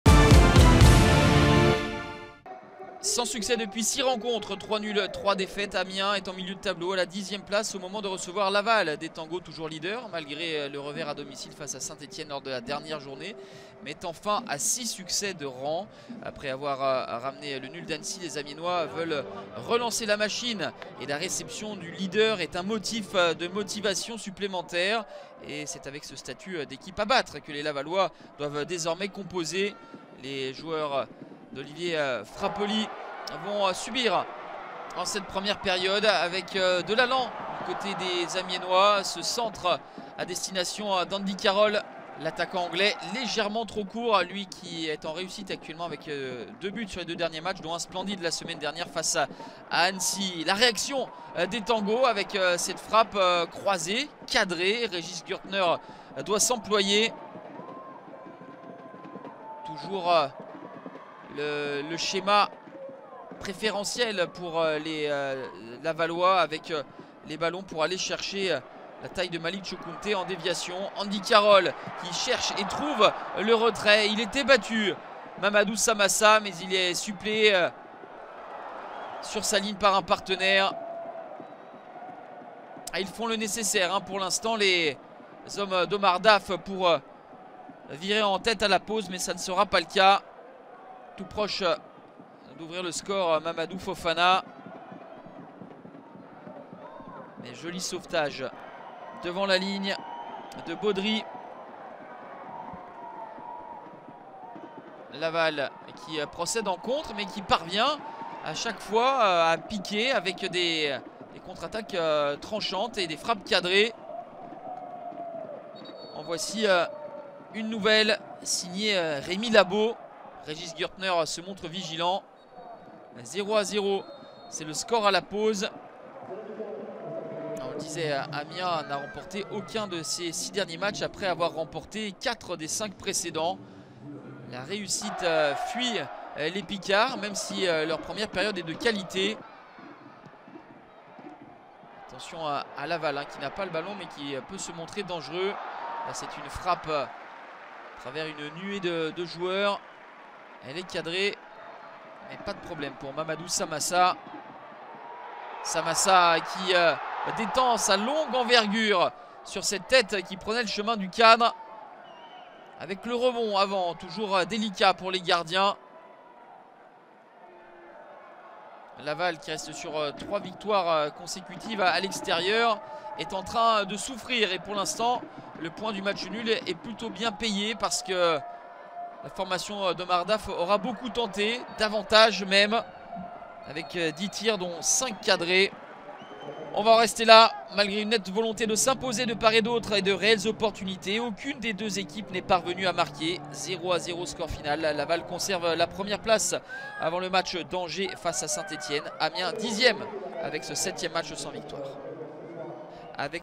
AMIENS SC - STADE LAVALLOIS MFC (0 - 0) - Résumé - (ASC - Laval) / 2023-2024
Revivez les meilleurs moments de AMIENS SC - STADE LAVALLOIS MFC (0-0) en vidéo.

Ligue 2 BKT - Saison 2023/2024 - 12ème journée STADE CRÉDIT AGRICOLE LA LICORNE - samedi 28 octobre 2023

Composition AMIENS SC : 1 - Régis GURTNER (c) /10 - Gaël KAKUTA /14 - Sébastien CORCHIA /24 - Jérémy GELIN /20 - Kylian KAÏBOUE /6 - Mamadou FOFANA /7 - Antoine LEAUTEY /2 - Mamadou FOFANA /4 - Nicholas OPOKU /9 - Louis MAFOUTA /99 - Andrew Thomas CARROLL

Composition STADE LAVALLOIS MFC : 1 - Régis GURTNER (c) /10 - Gaël KAKUTA /14 - Sébastien CORCHIA /24 - Jérémy GELIN /20 - Kylian KAÏBOUE /6 - Mamadou FOFANA /7 - Antoine LEAUTEY /2 - Mamadou FOFANA /4 - Nicholas OPOKU /9 - Louis MAFOUTA /99 - Andrew Thomas CARROLL